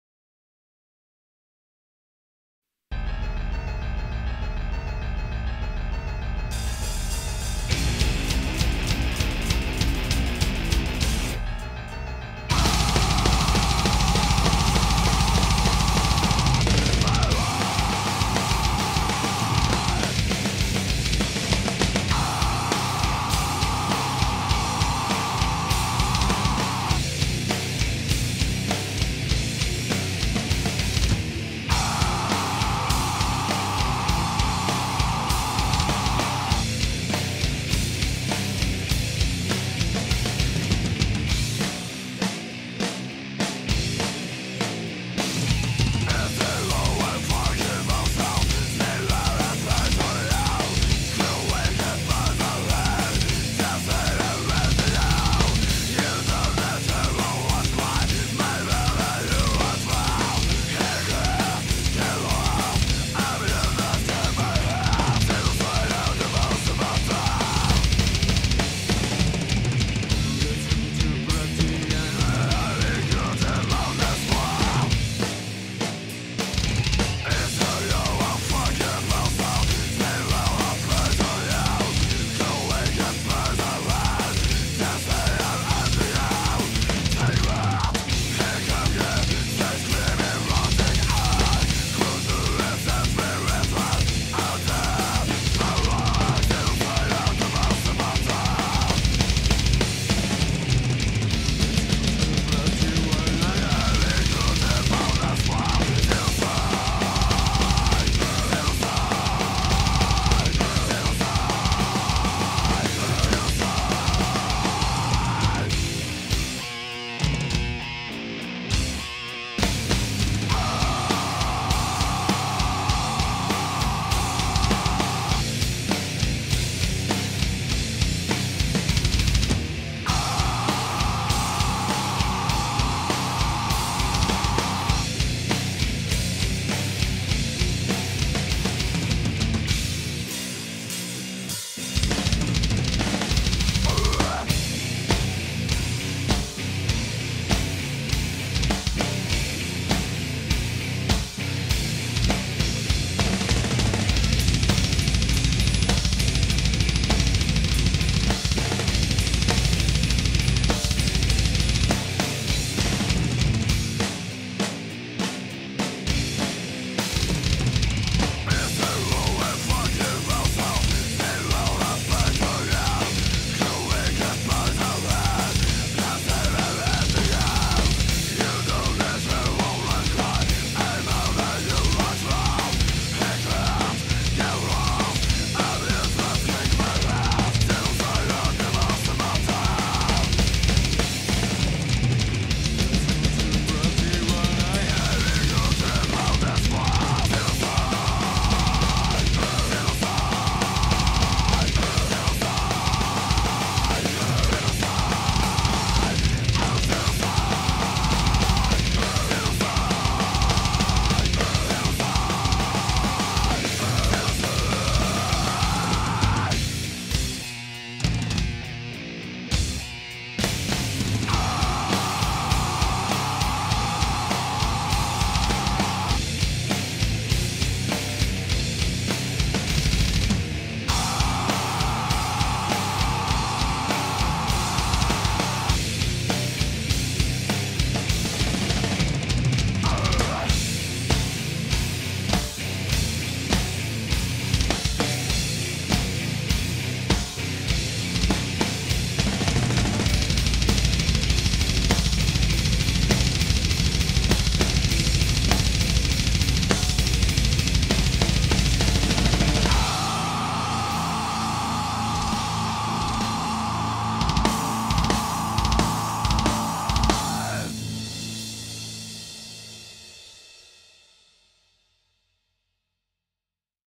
DEATHGAZE/あの名曲を本人が叩いてみた
訳あってタイトルは表示できませんが、ずっと撮りたかった曲です。
DEATHGAZEって感じです！！